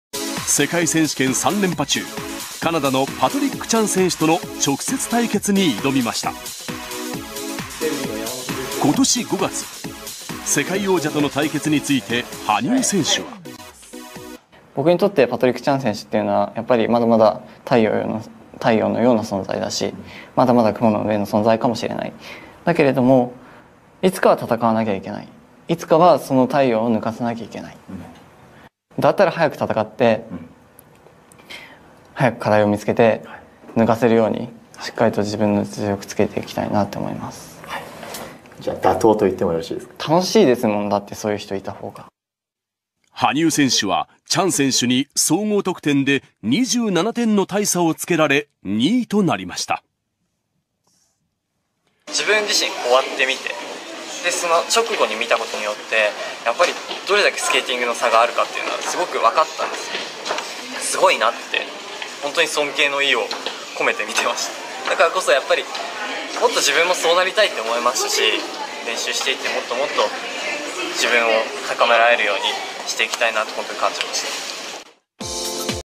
【羽生結弦 パトリック チャン】羽生結弦「楽しいですもん！そういう人いたほうが・・」
男子フィギュアスケート羽生結弦選手がパトリックチャン選手について語っています。
雲の上の人だからこそ、早く戦って課題を見つけたい・・
まじめな羽生選手らしいコメントです。